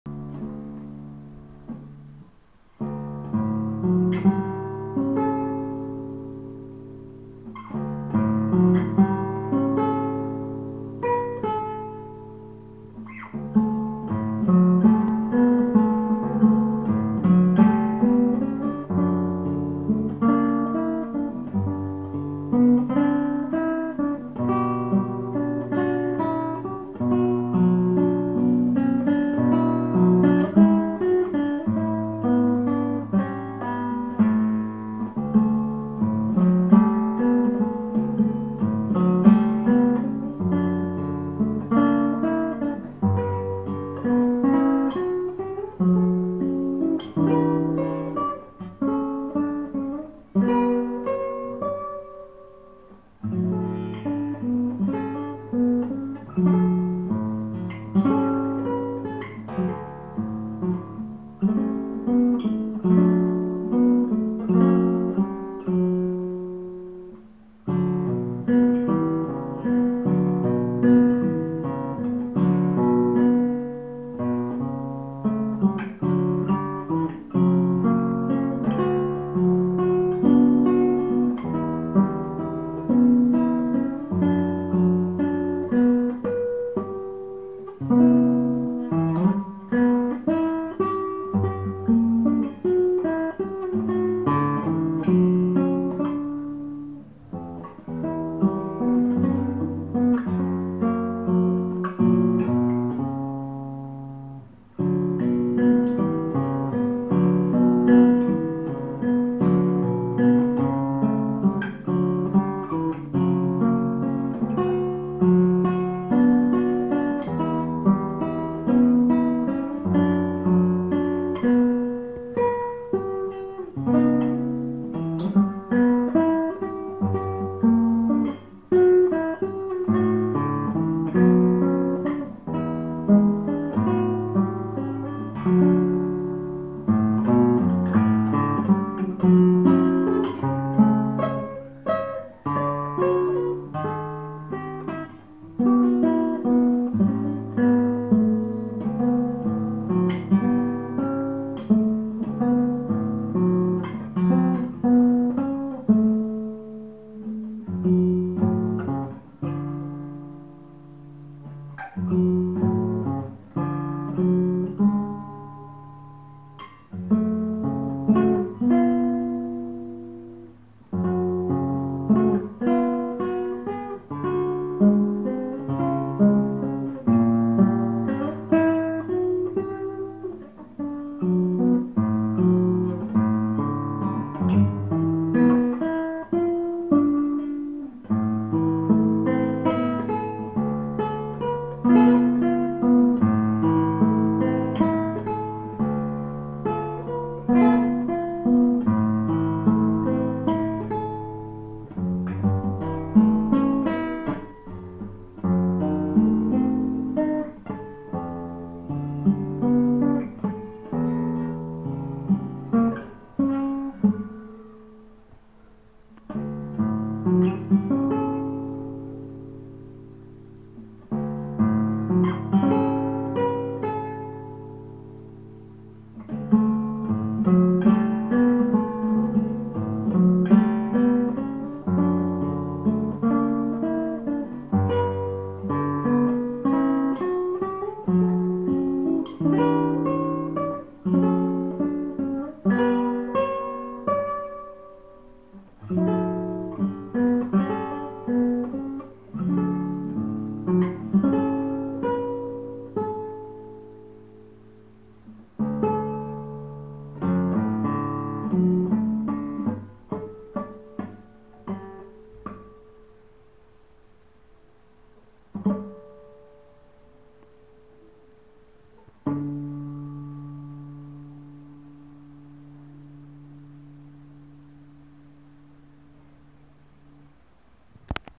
Guitarra Clasica Concierto Tarrago
Guitarra de concierto en abeto y jacaranda de India , construida por Carlos Espinosa Tarrago . Spruce top , East Indian Rosewoood B& S , Classical Guitar , handmade by Luthier Carlos Espinosa Tarrago . Patagonia Argentina